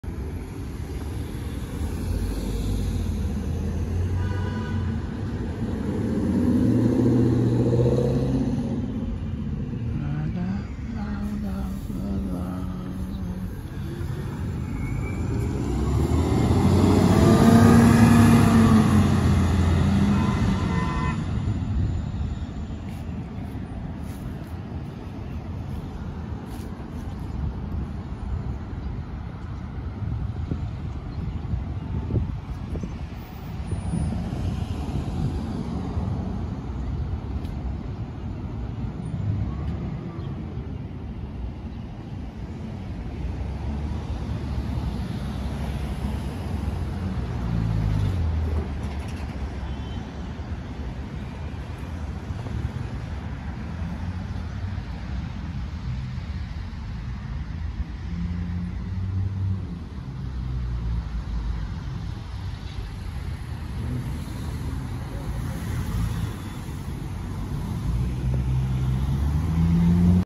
Queens, New York - Q47 Bus - Clean Air Hybrid Electric Bus
Q47 Bus just left the mall named Atlas Park in Queens, NY. Clean Air Hybrid Electric Bus. This is the first and last stops for that bus route.